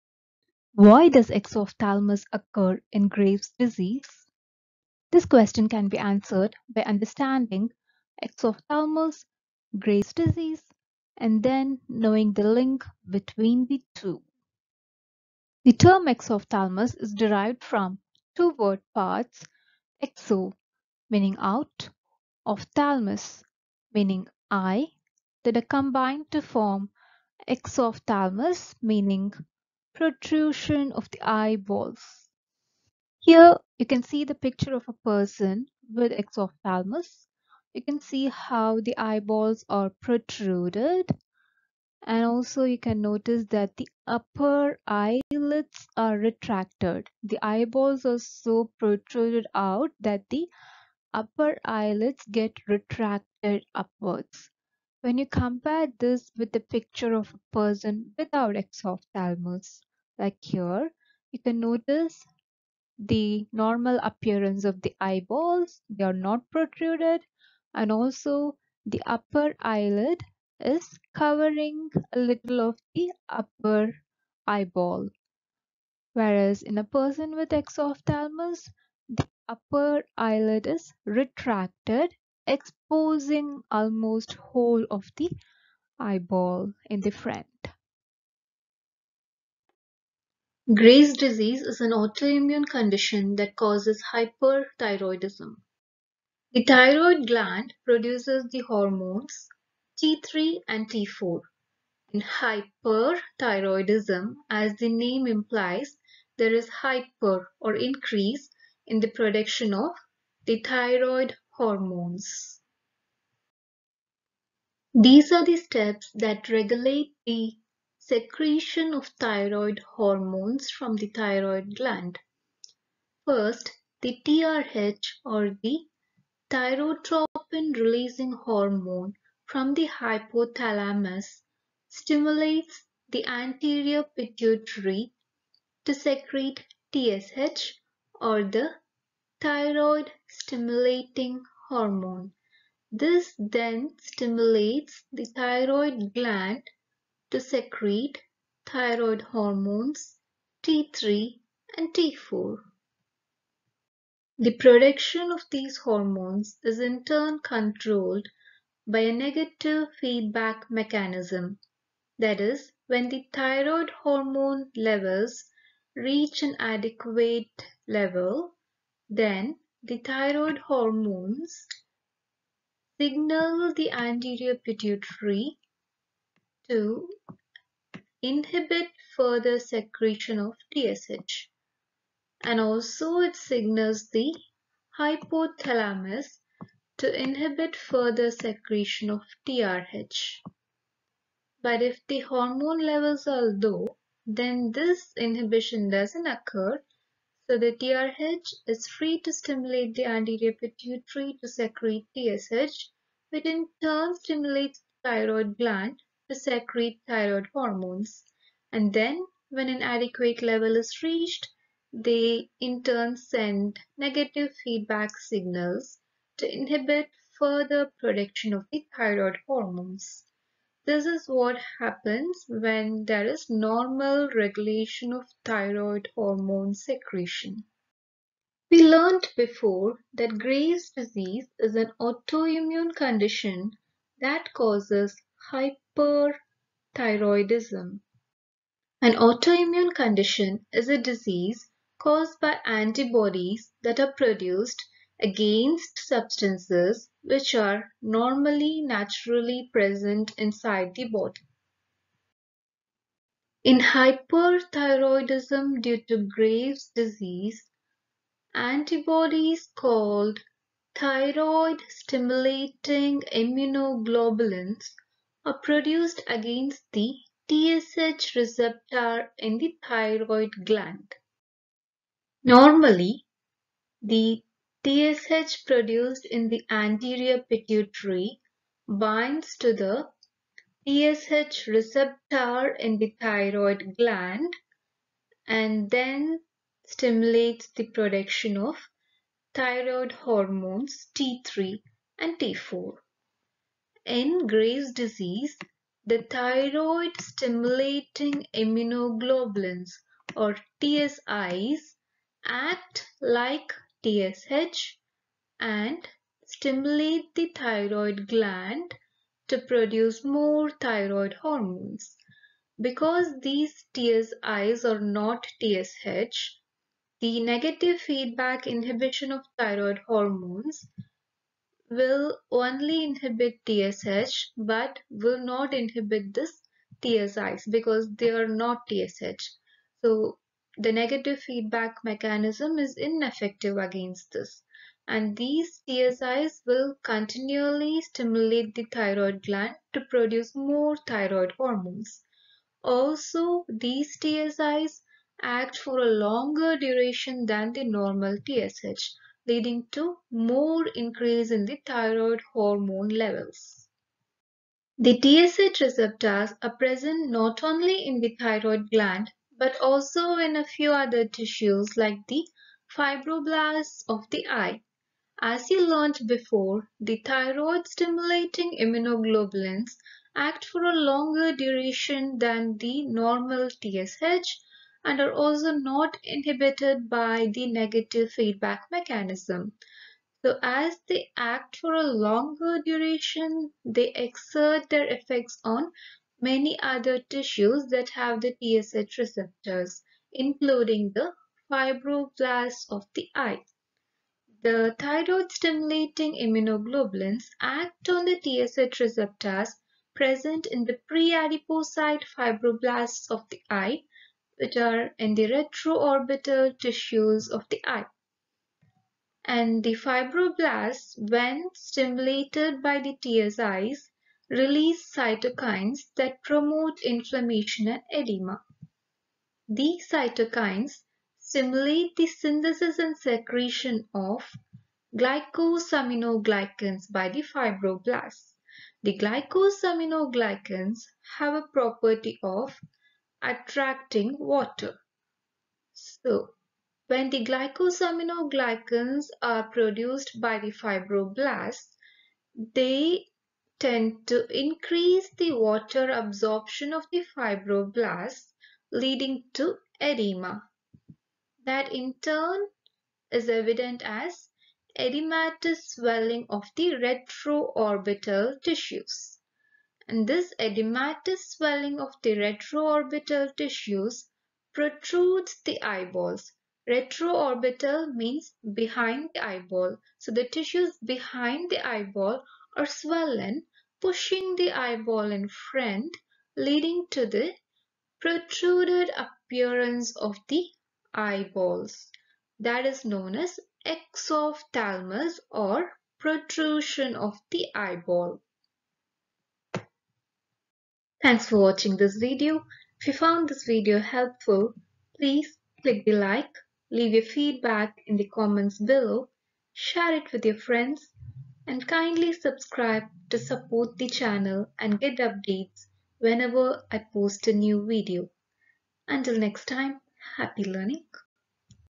Why does Exophthalmos occur in Graves’ Disease? What is Thyroid Eye Disease? Graves Ophthalmopathy
Cell Membrane MCQs Watch on 1.5x or 2x speed to save time:)
The term exophthalmos is derived from two root words, exo=out, ophthalmos=protrusion of the eyeball.
Graves' disease is an autoimmune cause of hyperthyroidism where TSIs act like TSH and increase thyroid hormone secretion.
TSIs or Thyroid Stimulating Immunoglobulins also exert effects on the preadipocyte fibroblasts of the eye. Due to the effects of TSIs, these fibroblasts release cytokines, which inturn stimulate production of glycosaminoglycans or GAGs that have water attracting properties. This leads to accumulation of water in fibroblasts of the eye, leading to edema.
Fibroblasts are more in the retroorbital tissues of the eye. This explains the retro-orbital edema seen in Graves' disease.
Retro-orbital (means behind the orbit) tissues are swollen leading to protrusion of the eyeballs.
This explains exophthalmos in Graves' disease.

Graves Orbitopathy
Graves Ophthalmopathy
Thyroid Eye Disease
Thyroid Oculopathy

Timestamps
0:00 Introduction
0:15 Exophthalmos
1:30 Graves' Disease
5:54 Exophthalmos in Graves’ Disease